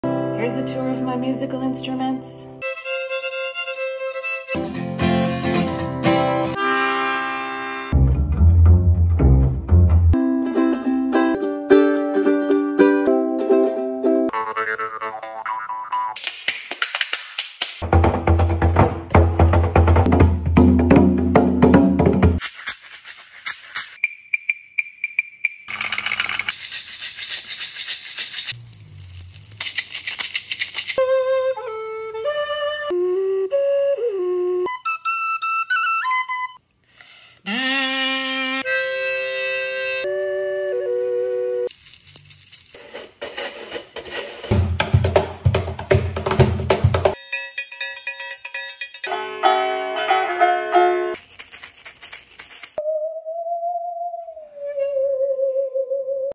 Musical Instrument Show-and-Tell
Here's a quick tour of my musical instruments.  This video was really fun to make, by the way. ;-)

In order of appearance:
Guitar
Fiddle
Guitar
Melodica
Bass
Ukulele X 3
Jaw Harp
Musical Spoons
Bodhran
Ashiko Drum
Caxixi
Clave
Assorted Shakers
Alto Recorder
Ocarina
Pennywhistle
Kazoo
Harmonica
Ocarina
Shaker Bells
Washboard
Cajon
Triangle
Strum Stick
Shaker Eggs
Musical Saw